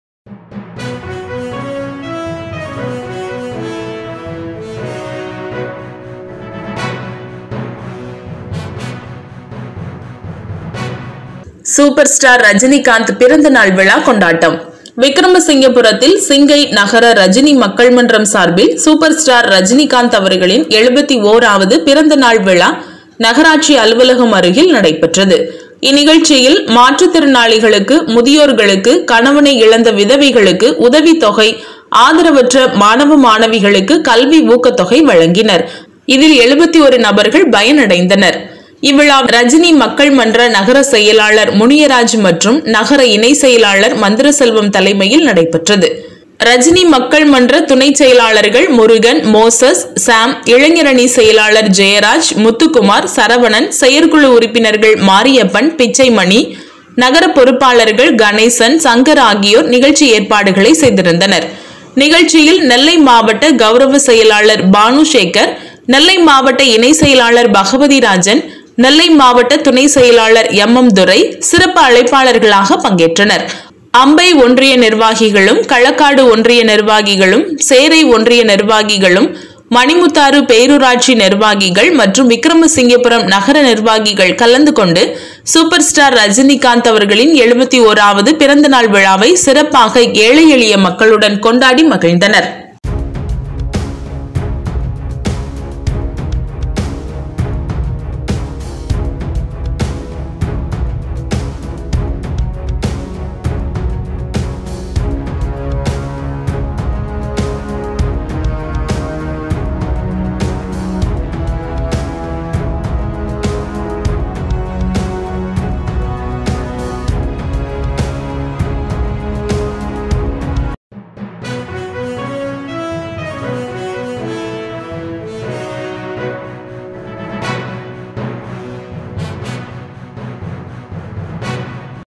சூப்பர் ஸ்டார் ரஜினிகாந்த் பிறந்த நாள் விழா கொண்டாட்டம்
சிங்கை நகர ரஜினி மக்கள் மன்றம் சார்பில் சூப்பர் ஸ்டார்  ரஜினிகாந்த் அவர்களின் 71வது பிறந்தநாள் விழா நகராட்சி அலுவலகம் அருகில் நடைபெற்றது.

இந்நிகழ்ச்சியில் மாற்று திறனாளிகளுக்கு, முதியோர்களுக்கு,கணவனை இழந்த விதவைகளுக்கு உதவி தொகை,ஆதரவற்ற மாணவ, மாணவிகளுக்கு கல்வி ஊக்கத்தொகை வழங்கினர், இதில் 71 நபர்கள் பயணடைந்தனர்.

 இவ்விழாவில் ரஜினி மக்கள் மன்ற நகர செயலாளர் வி. முனியராஜ் மற்றும் நகர இணைச் செயலாளர் மந்திர செல்வம், தலைமையில் நடைபெற்றது

 ரஜினி மக்கள் மன்ற துணைச் செயலாளர்கள் முருகன்,மோசஸ் ,ஷேம், இளைஞர் அணி செயலாளர்,ஜெயராஜ்,
முத்துக்குமார்,சரவணன்,செயற்குழு உறுப்பினர்கள்  மாரியப்பன்,பிச்சைமணி,நகர பொறுப்பாளர்கள் திரு கணேசன்,சங்கர் ஆகியோர் நிகழ்ச்சி ஏற்பாடுகளை செய்திருந்தனர்.

நிகழ்ச்சியில் நெல்லை மாவட்ட கௌரவ செயலாளர் த.பானு சேகர்,நெல்லை மாவட்ட இணைச் செயலாளர் பகவதி ராஜன்,
 நெல்லை மாவட்ட துணை செயலாளர் எம் எம் துரை,சிறப்பு அழைப்பாளர்களாக பங்கேற்றார்கள்.
அம்பை ஒன்றிய நிர்வாகிகள் ஒன்றிய நிர்வாகிகளும் களக்காடு ஒன்றிய நிர்வாகிகள்,சேரை ஒன்றிய நிர்வாகிகள், நிர்வாகிகளும், மணிமுத்தாறு பேரூராட்சி நிர்வாகிகளும், மற்றும் விக்கிரமசிங்கபுரம் நகர நிர்வாகிகள் கலந்து கொண்டு சூப்பர் ஸ்டார் ரஜினிகாந்த் 71வது பிறந்தநாள் விழாவை சிறப்பாக ஏழை எளிய மக்களுடன் கொண்டாடி மகிழ்ந்தனர்.